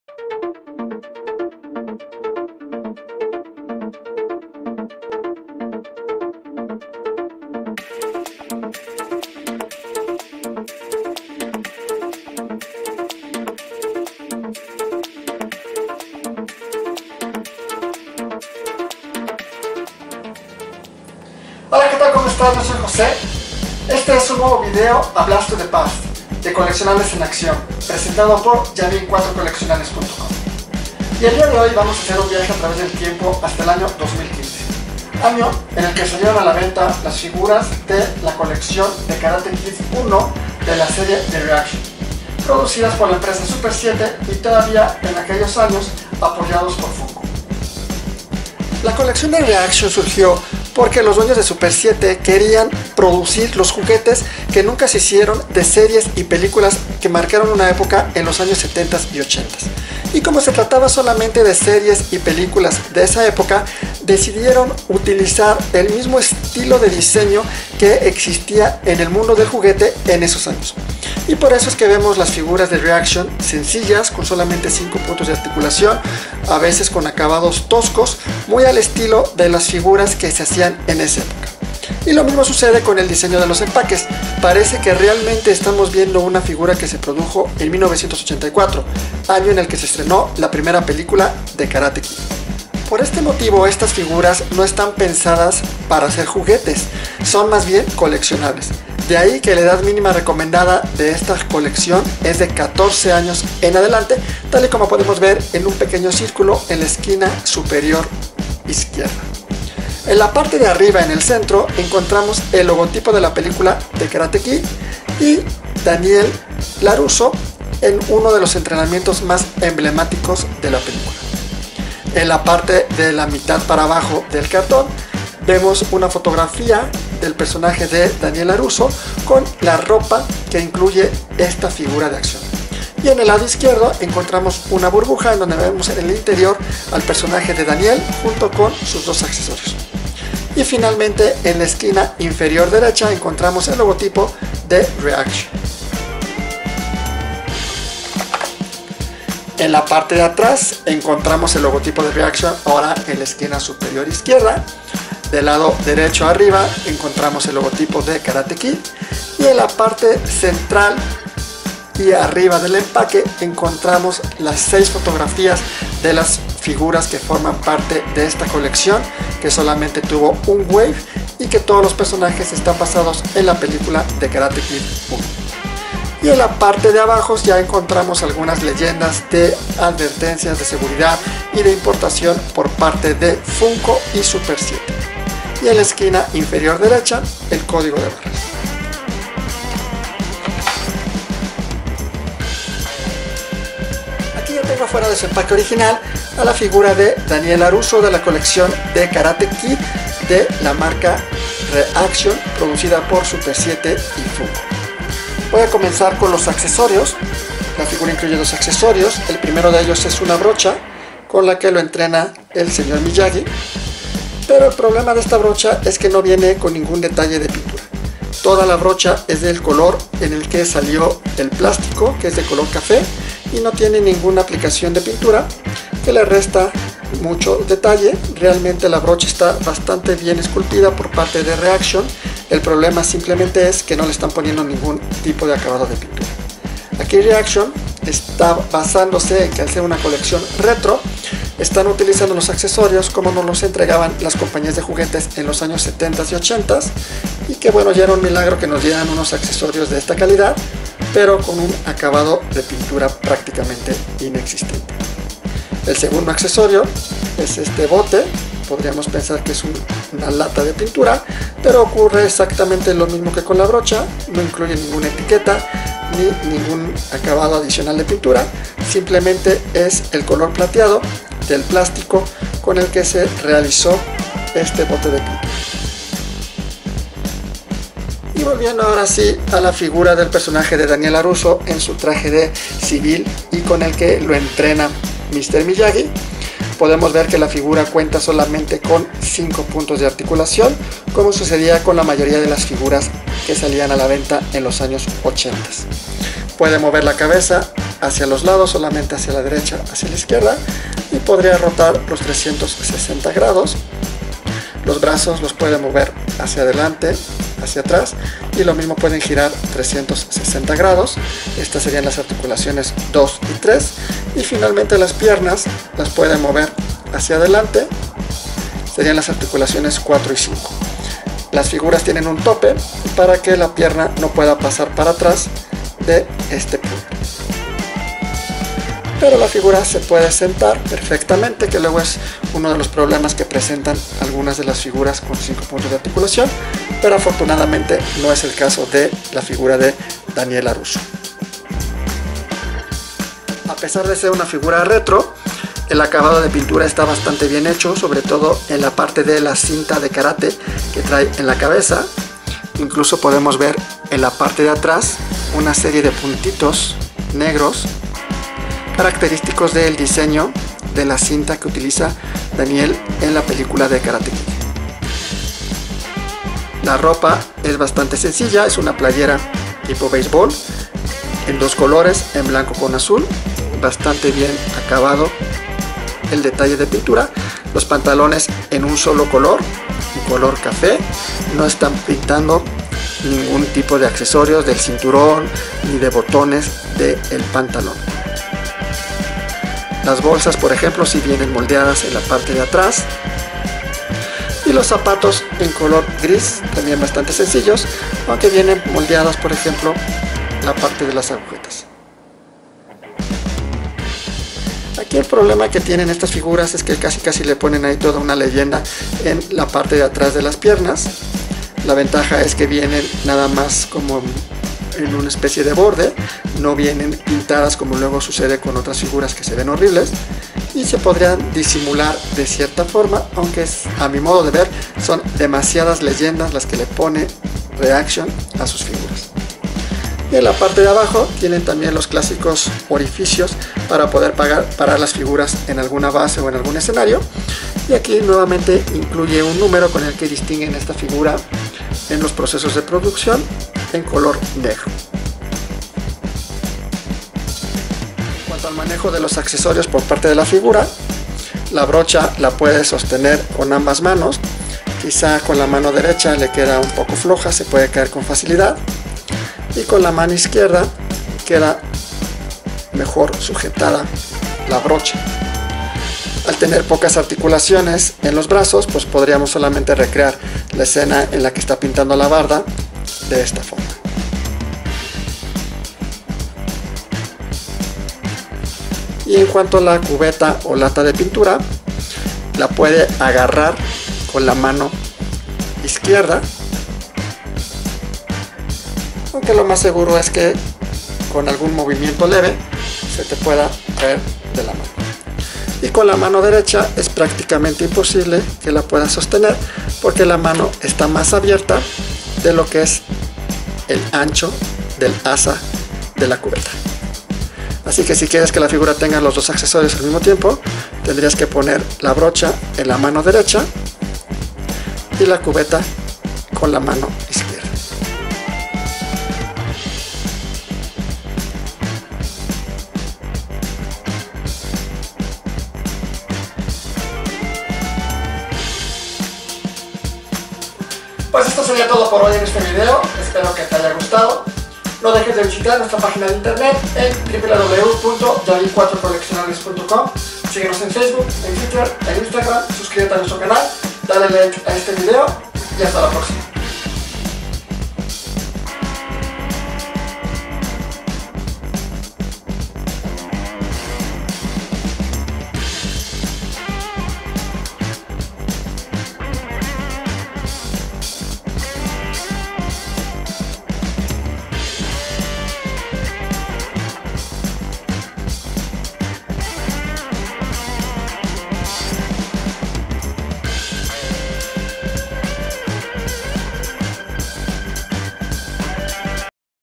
Blast to the Past: Review Daniel Laruso- Karate Kid- Reaction- 3 3/4"
Revisión de la figura de Reaction de tamaño 3 3/4" de Daniel Laruso de la película Karate Kid